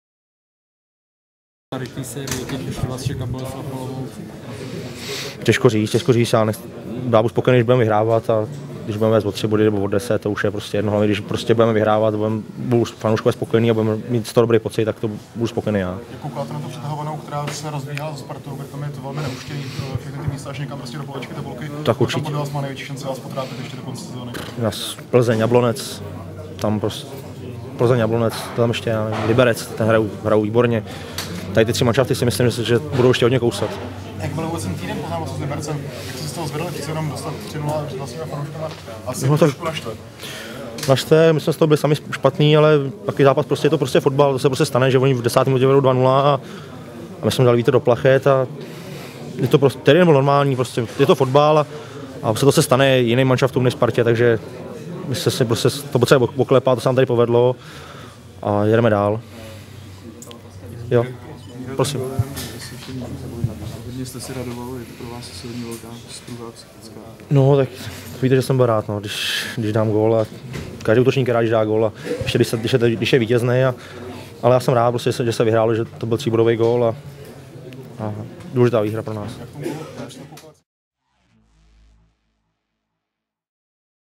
Miroslav Slepička po utkání v Teplicích
Rozhovor s útočníkem AC Sparta Praha Miroslavem Slepičkou po utkání FK Teplice - ACS 0:1. Více info na